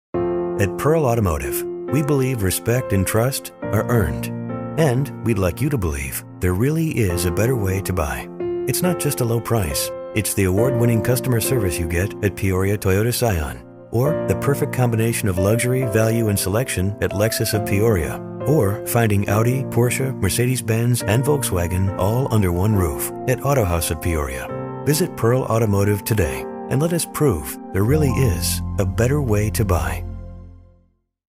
Welcome to Pearl Automotive!
Family-owned Pearl Automotive has provided quality new and pre-owned automobiles to Central Illinois for nearly 35 years. Jack Pearl founded this world-class organization on the principle of offering only the most reliable cars and helping consumers maintain their investments through top-of-the-line parts and service.